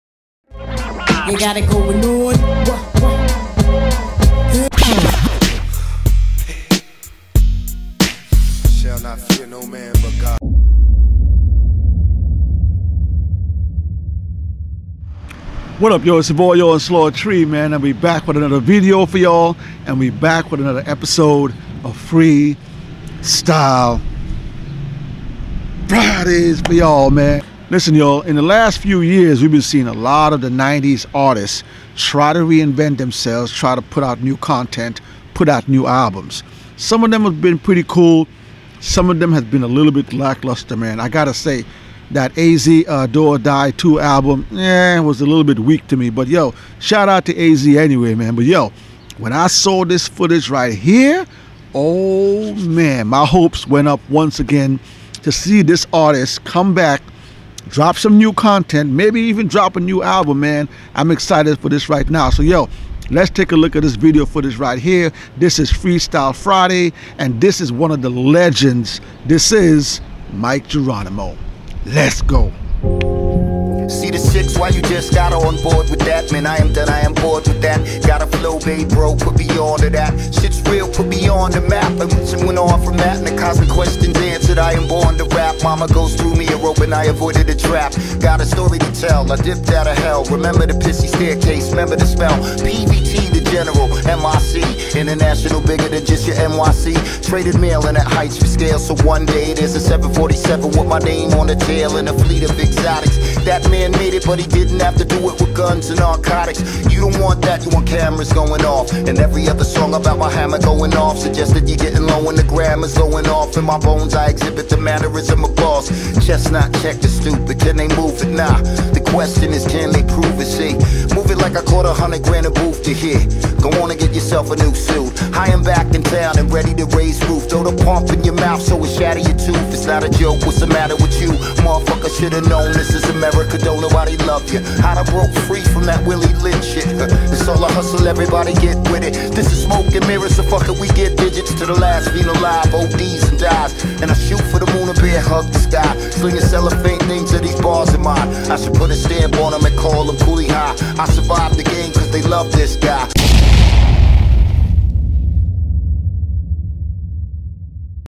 Mic Geronimo is BACK!! 2023 Freestyle GOES BONKERS
We appreciate you all.
Wow, I did not expect Mic Geronimo to go so bonkers on this freestyle.